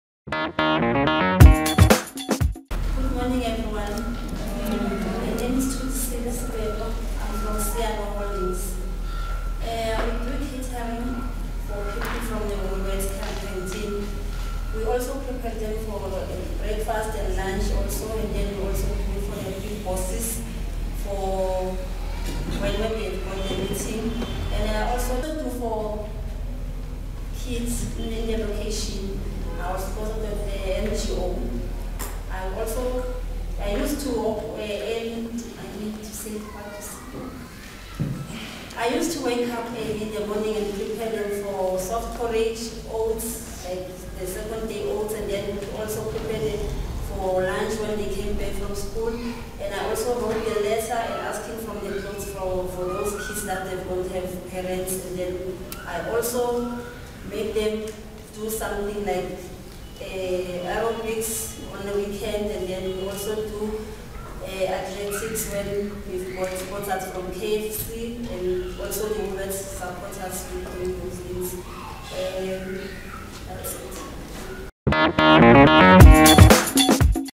Knorrox Kitchen Queens Program
Knorrox Kitchen Queen, Thulisile Sibeko, from Naledi, Soweto, doing her two minute presentation.